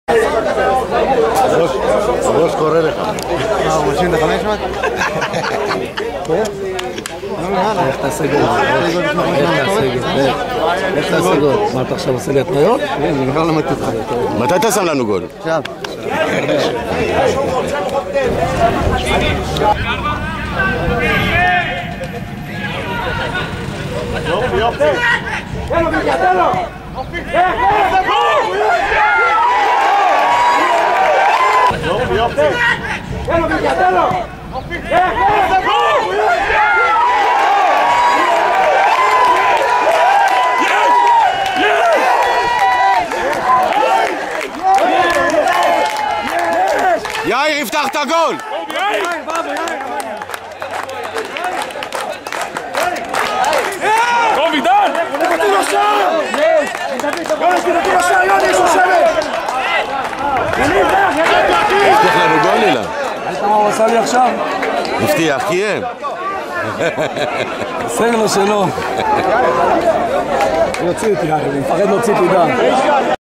מכבי קביליו יפו נגד מרמורק גביע הגול של יאיר
מישחק גביע יאיר מבטיח גול במחצית ומקים